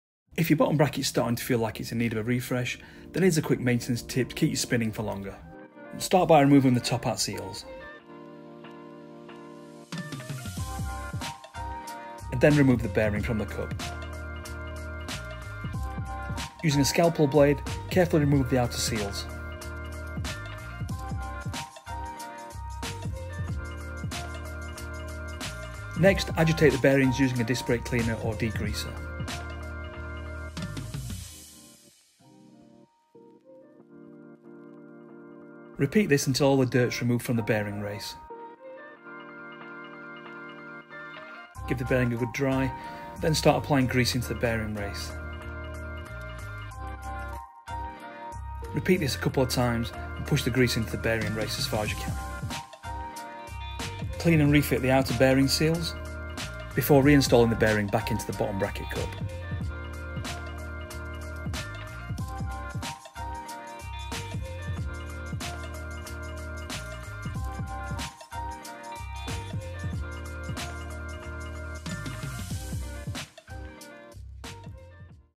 This Tip Will Make Your Bottom Bracket Last Longer
Make your bottom bracket last longer with this simple maintenance tip🔧